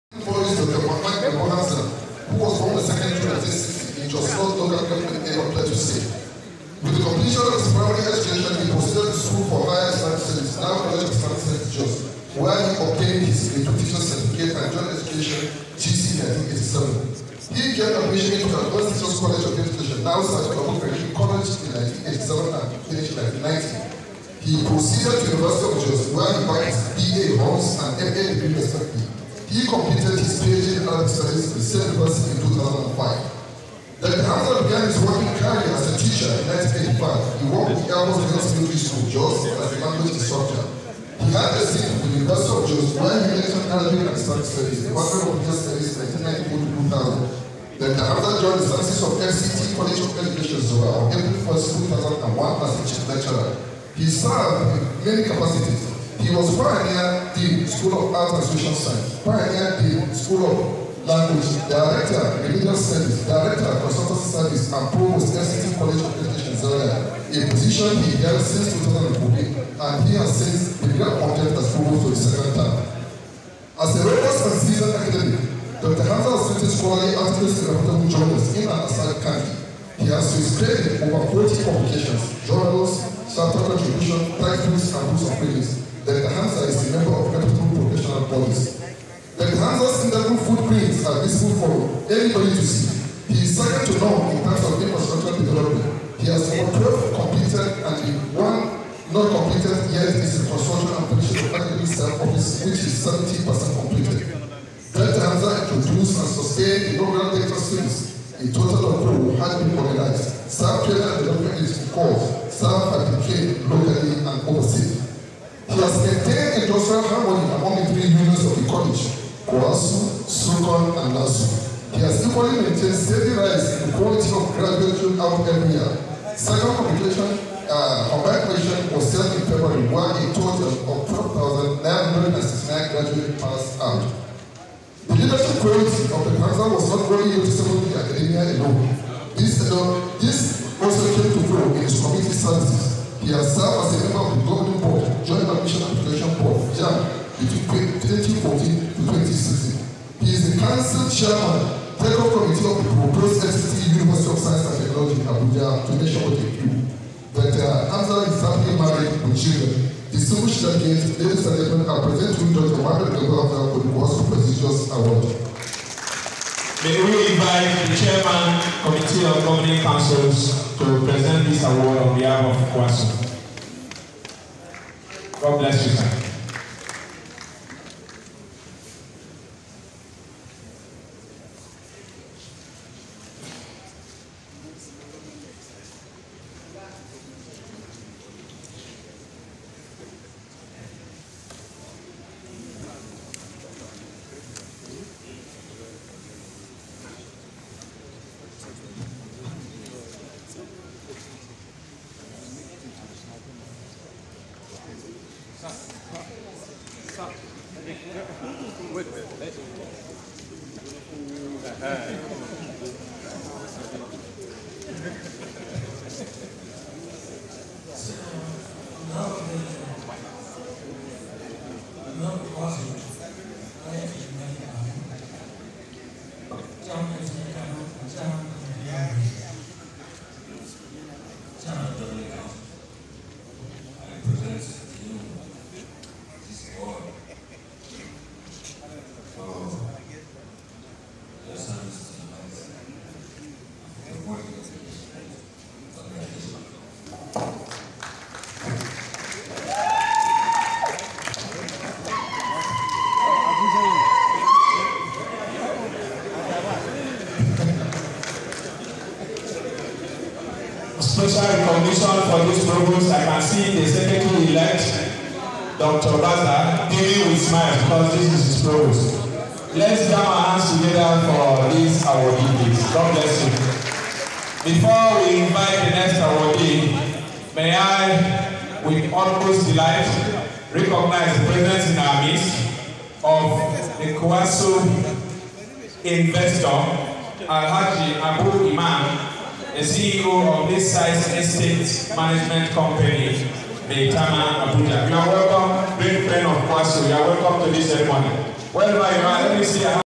Astute Administrator Award Conferred on Dr. Muhammad G. Hamza. Provost, FCT COE Zuba By COEASU.
Dr. Muhammad Garba Hamza is the current FCT College of Education Zuba provost. Until his appointment, Dr. Muhammad Gambo Hamza, was a Chief Lecturer and the Director, Directorate of Continuing Education at the college.

The current provost, who holds a doctorate degree in Arabic Studies from the University of Jos, began his working career as a graduate assistant in 1993 before transferring his service to the Education Resource Centre, Federal Capital Territory Administration in 1997.

He joined the services of the FCT College of Education, Zuba in 2000 as chief lecturer and has held other positions prior to his appointment as provost which includes Dean, School of Arts and Social Science, Dean, School of Languages, Director, Directorate of Remedial Studies, Director, Consultancy Services and Director, Directorate of Continuing Education.

Dr. Hamza, an author of several academic works, is also a member of several academic and professional bodies including the Nigerian Association of Study of Religions, Association of Teachers of Arabic and Religious Studies and the National Board of Arabic and Islamic Studies.